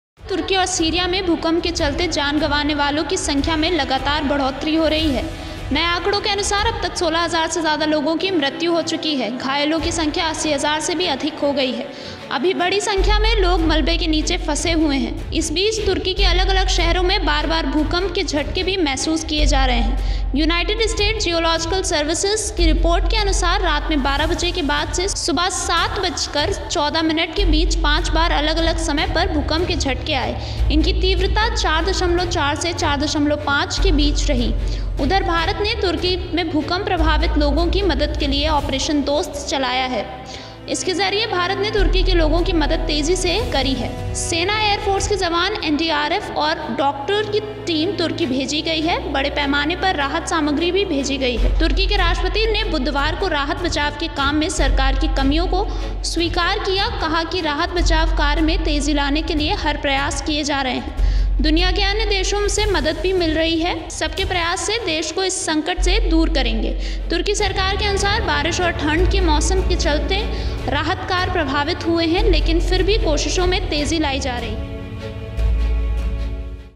Turkey-Syria Earthquake: भूकंप से मरने वालों का आंकड़ा 16 हजार के पार, भारत ने चलाया ऑपरेशन 'दोस्त'
तुर्किये और सीरिया में भूकंप के चलते जान गंवाने वालों की संख्या में लगातार बढो़तरी  हो रही है। नए आंकड़ों के अनुसार, अब तक 16 हजार से ज्यादा लोगों की मृत्यु हो चुकी है। घायलों की संख्या 80 हजार से भी अधिक हो गई है। अभी बड़ी संख्या में लोग मलबे के नीचे फंसे हुए हैं।

 भारत ने तुर्किये में भूकंप प्रभावित लोगों की मदद के लिए 'ऑपरेशन दोस्त' चलाया है। इसके जरिए भारत ने तुर्किये के लोगों की मदद तेज कर दी है। सेना, एयरफोर्स के जवान, एनडीआरएफ ऑर डॉक्टर्स की टीम तुर्किये भेजी गई है। बड़े पैमाने पर राहत सामग्री भी भेजी गई है।